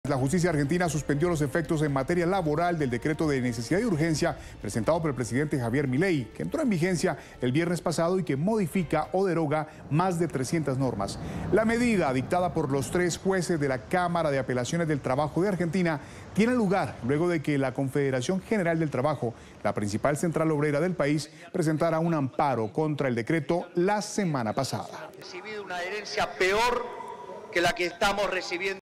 Reforma laboral que impulsa Javier Milei fue suspendida cautelarmente
La decisión de la Cámara Nacional del Trabajo ocurre después de las manifestaciones registradas a finales de 2023 que buscan frenar el decreto de necesidad y urgencia promovido por el mandatario derechista.